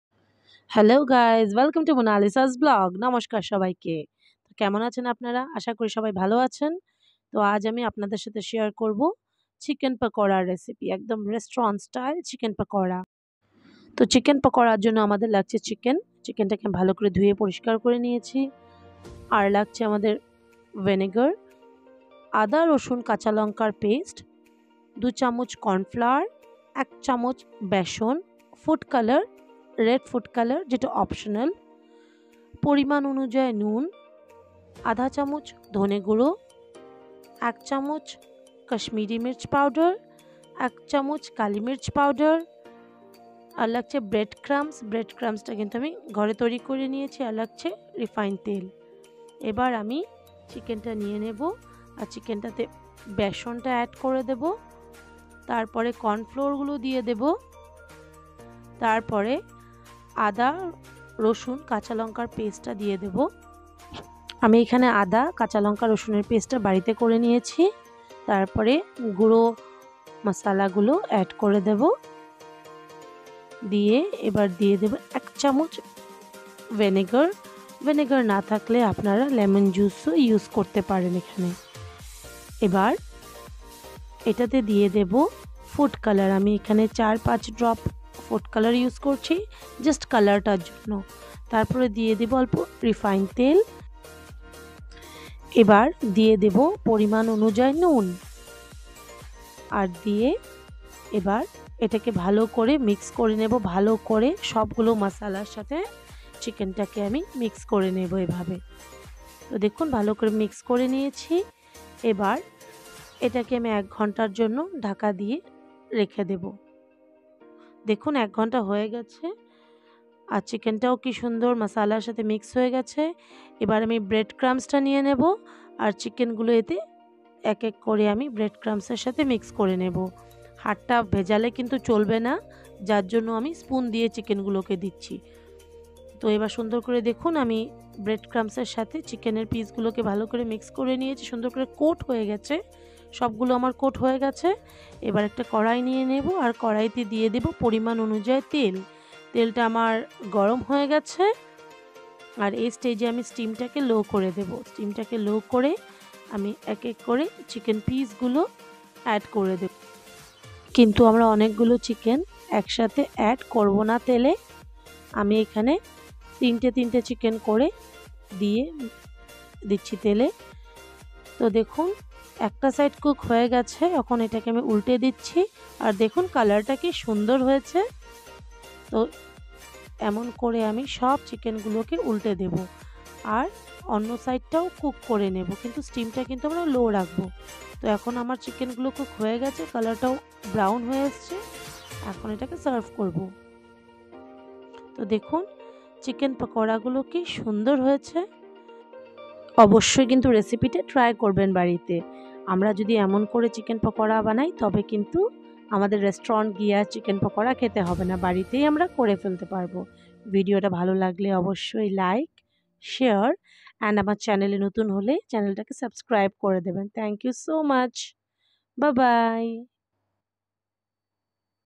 How to make Restaurant style Chicken Pakoda at home||Chicken Pakoda Recipe
Chicken Pakoda is a famous street side food in which chicken is marinated with spices, coated with bread crumbs and deep fried in oil.It can be prepared at home even